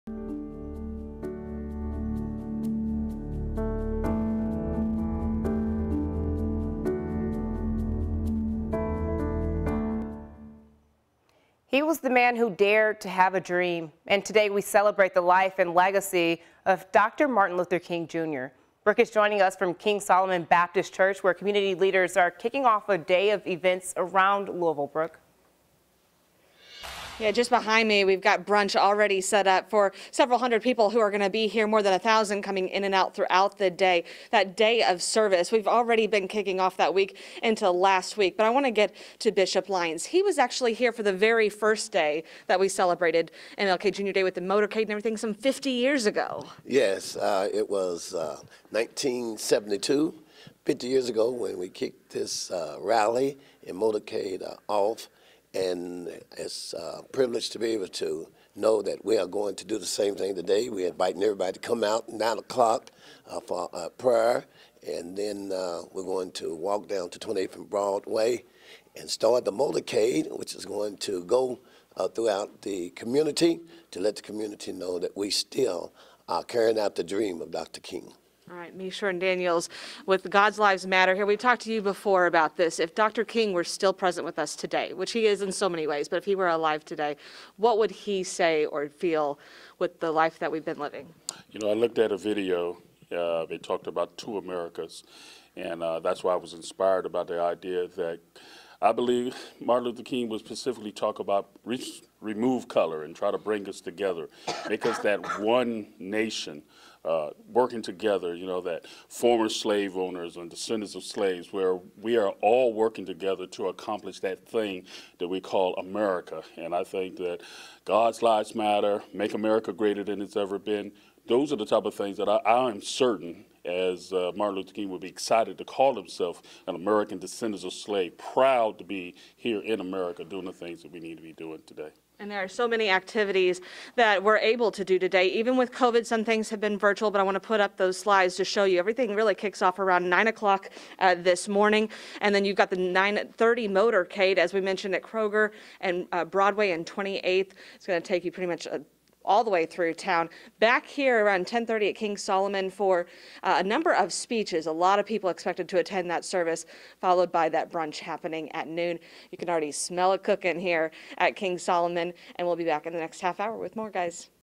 How the legacy of Martin Luther King, Jr. is inspiring community service in Louisville
Bishop Dennis Lyons of the Voice of Louisville and Meshorn Daniels with God's Lives Matter share their reasons for coming together on MLK Day.